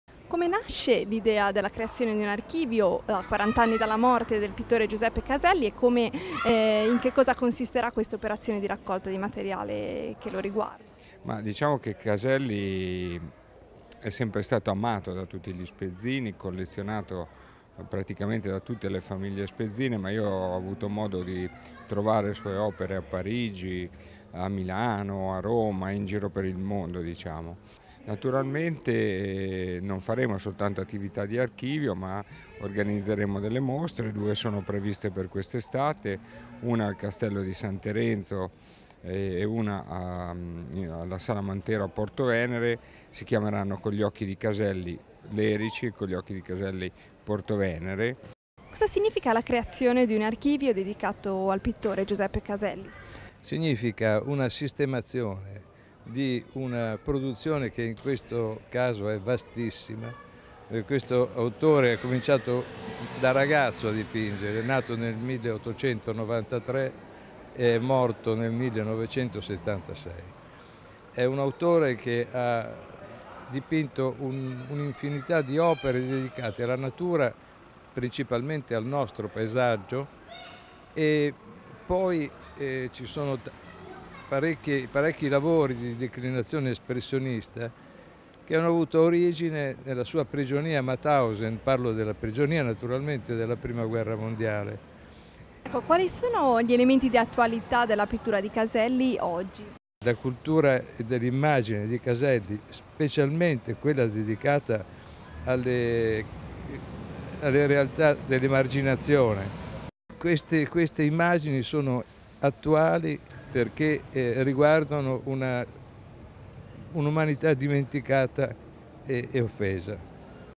presentato l''archivio Caselli 28-05-2016
Presentato questa mattina all’Urban center di via Manin, alla Spezia, l’archivio Giuseppe Caselli, che l’associazione culturale Startè ha deciso di realizzare a 40 anni di distanza dalla scomparsa del pittore amatissimo dagli spezzini e conosciuto in tutto il mondo.
A presiedere il Comitato scientifico dell’archivio, l’allievo Francesco Vaccarone che conobbe Caselli nel ’56 all’età di 16 anni.